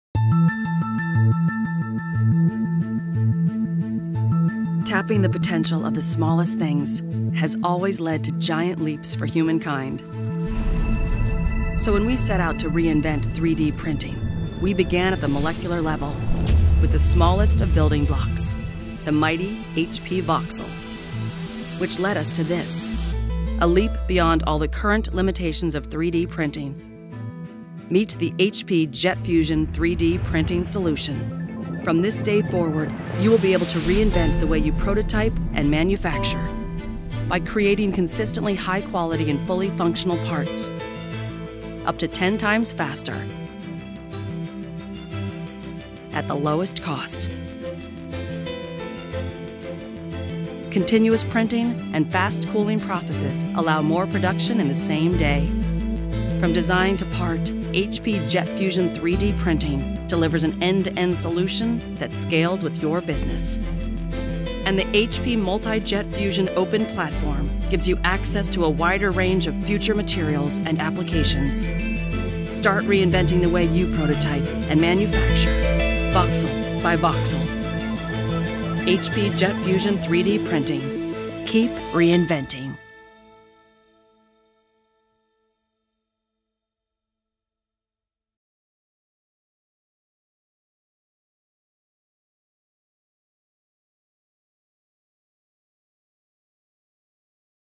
HP 3D Multi Jet Fusion Printing Reinventing Manufacturing
In 2018 Matsuura UK further enhanced our 3D printing and Additive Manufacturing product portfolio by becoming a reseller for HP Multi-Jet Fusion printers. This astounding technology will change the way you think about 3D plastics printing - be that for prototyping, short runs or mass production. As with all our product brand partners; Matsuura UK will fully support our HP customers with Technical and long term Applications support from our Leicestershire HQ.